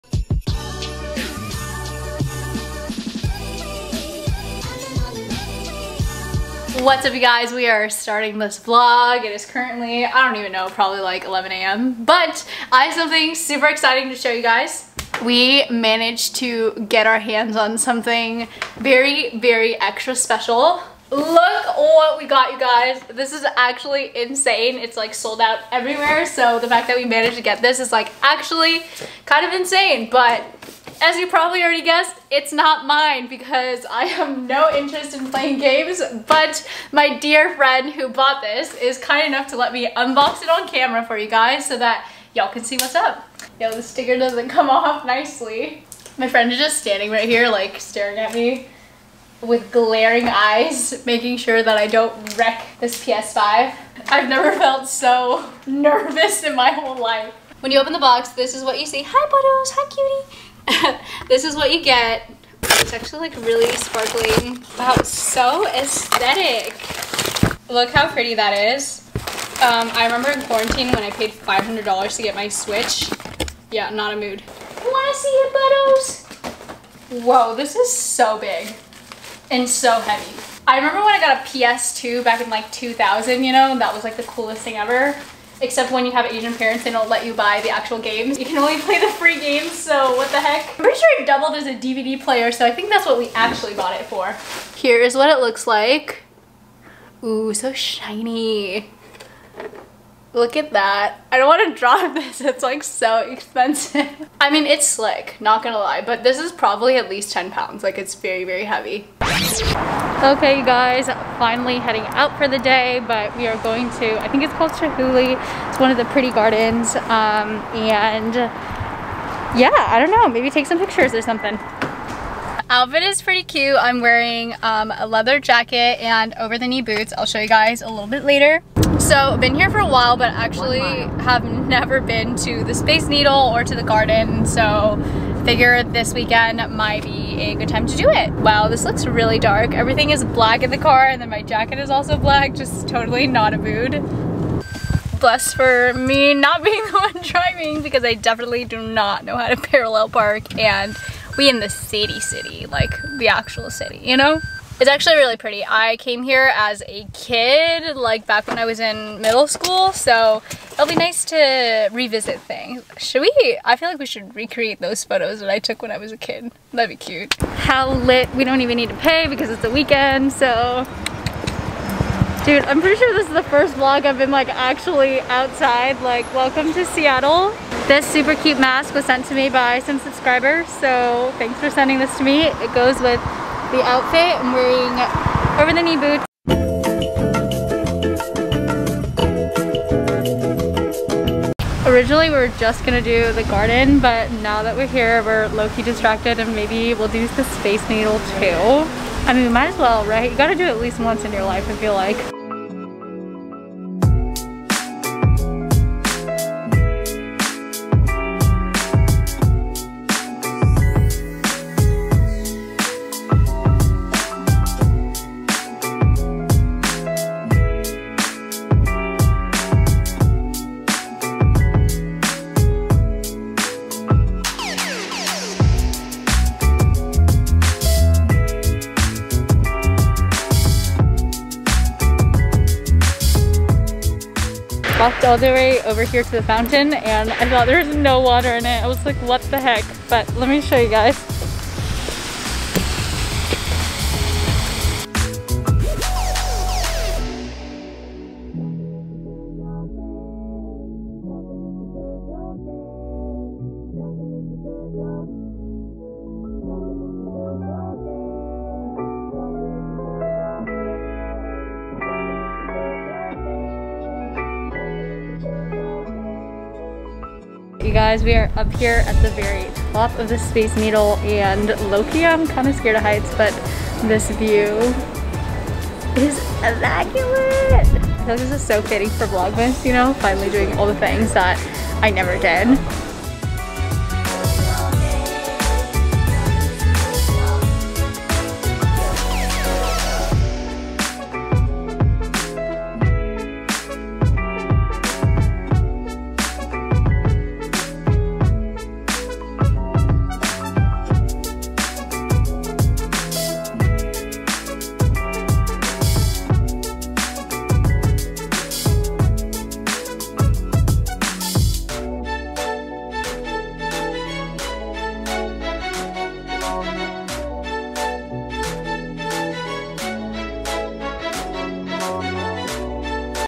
UNBOXING THE PS5 + Exploring Seattle (space needle) | vlogmas day 6
And in the blink of an eye we are on day 6 already!!
I'm very rarely a tourist in my own city so hope you enjoyed the virtual tour :)
𝘀𝐞𝗻𝗱 𝘂𝘀 𝗺𝗮𝗶𝗹
Miki and Kevin